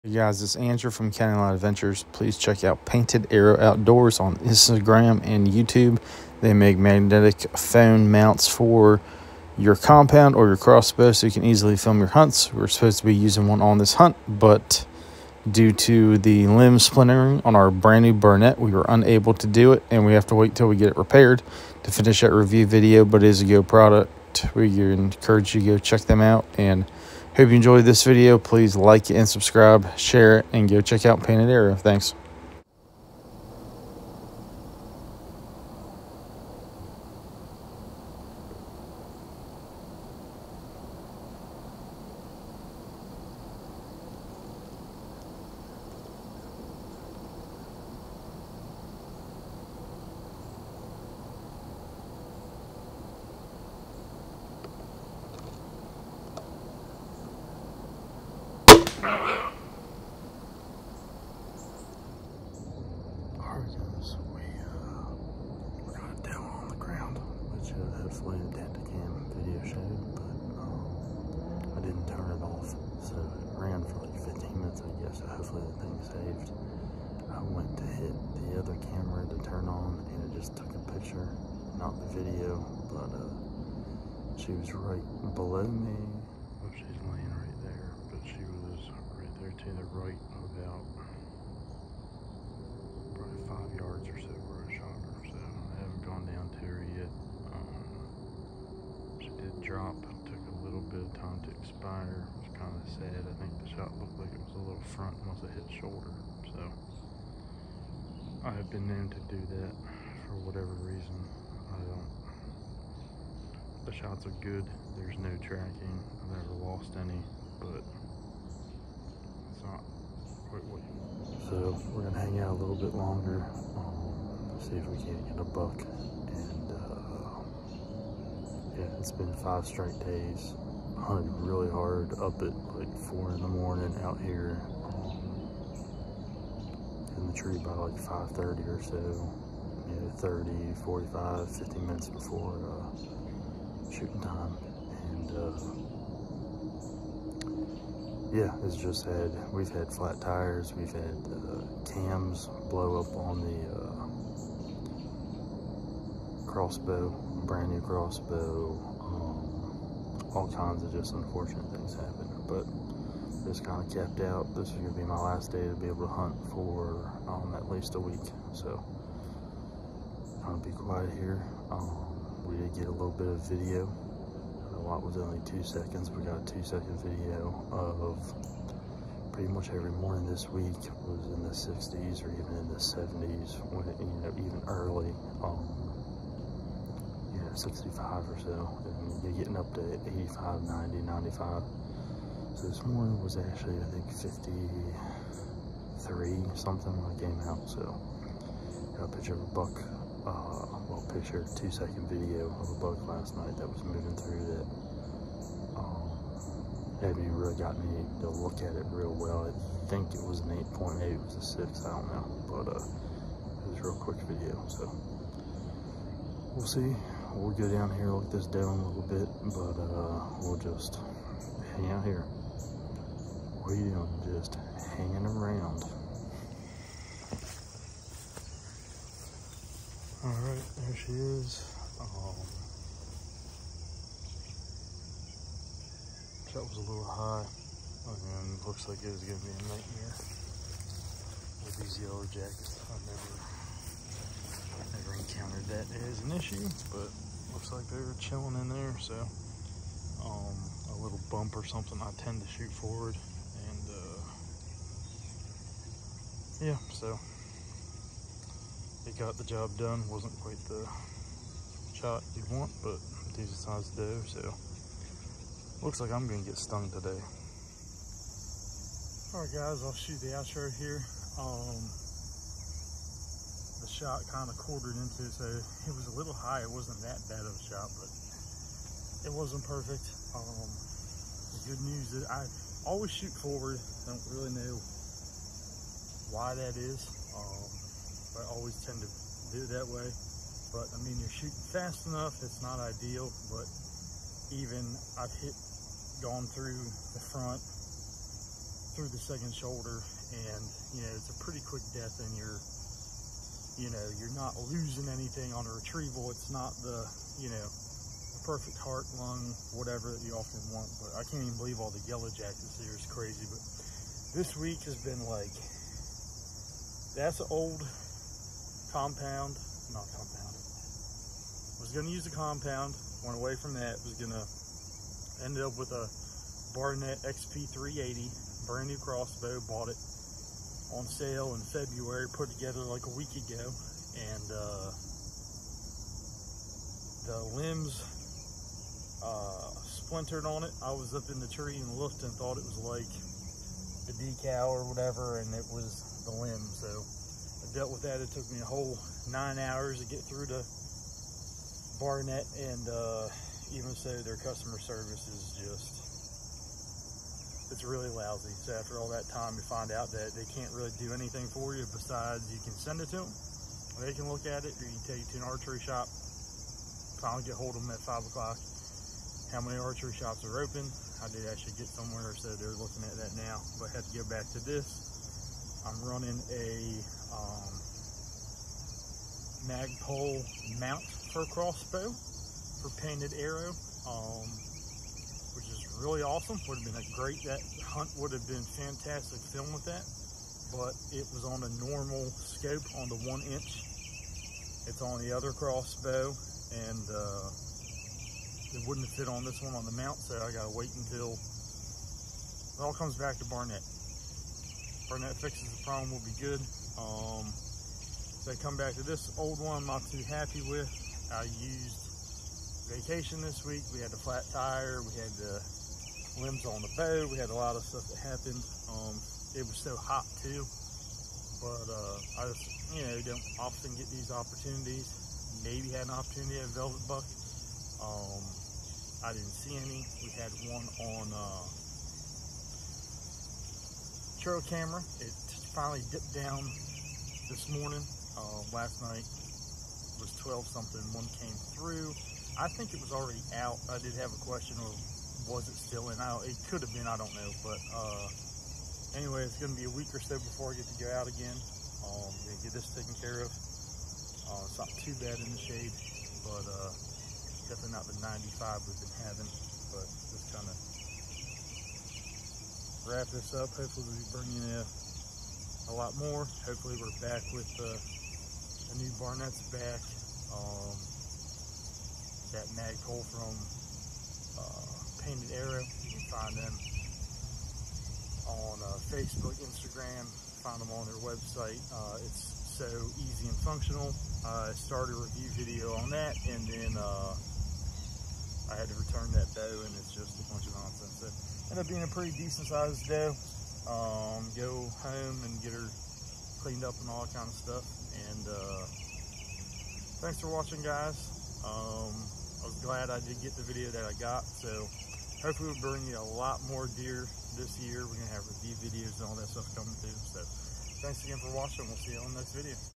First Doe of 2023 Season- Brand New@barnettcrossbows XP 380 Limbs Splintered so Backup Plan 🤦‍♂️
We took off five days to hunt the early season with hopes of getting a velvet buck. We had people mowing at the property, homeless people, flat tires, hot weather Monday & Tuesday then on Wednesday we noticed the limbs on our brand new @barnettcrossbows we’re splintered so we spent from 8:45AM- 4:50pm to get a hold of them and they would not send us a new crossbow or the new limbs to complete our filming. We are extremely disappointed with how their bows failure was handled by their customer service.
We explained our filming and testing of the @paintedarrowoutdoors Mag Pro X and wanting to continue testing on our vacation days but they did nothing to help us.

So apologies to the @paintedarrowoutdoors guys as we wait for the return of our XP 380 so we can finish their video. They provide a magnetic based mounting system for your cell phone to capture your hunt! We were extremely excited with setting it up, we think it will be helpful to you guys! We’ll get those videos up asap. Sorry guys!

Tell them @CountyLineAdventures  sent you.

This was the first filled tag of 10+ that we have this season so please stick with us as we continue to bring you hunts & new products.